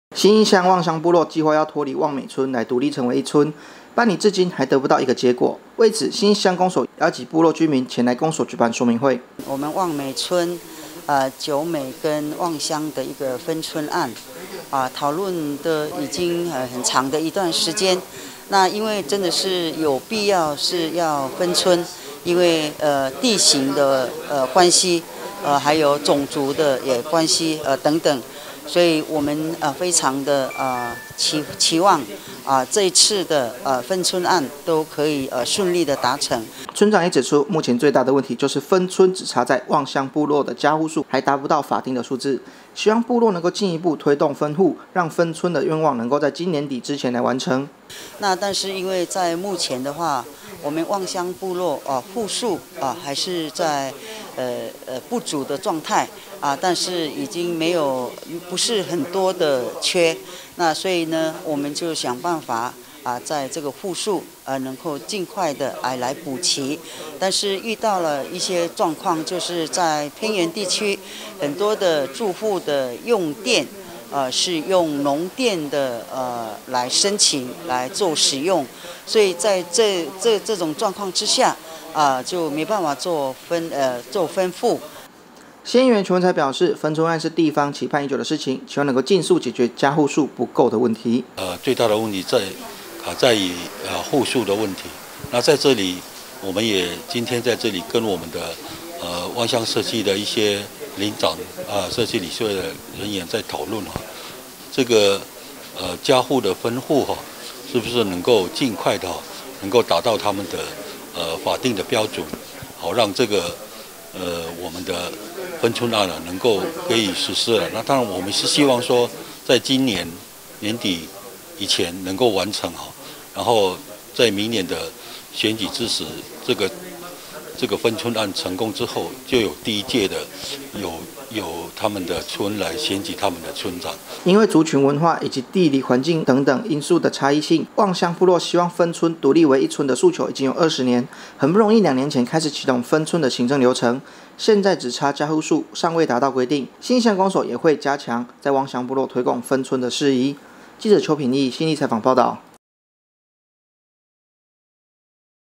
20210827 民議新聞 望美分村案差在望鄉家戶數不足 盼相關單位儘速協助(縣議員 全文才)
信義鄉望鄉部落，計畫要脫離望美村來獨立為一村，辦理至今還得不到一個結果。為此，信義鄉公所邀集部落居民，前來公所舉辦說明會，強調目前分村只差在望鄉部落的家戶數還不到法定的數字，希望部落能夠進一步推動分戶，讓分村的願望能在今年底之前來實現。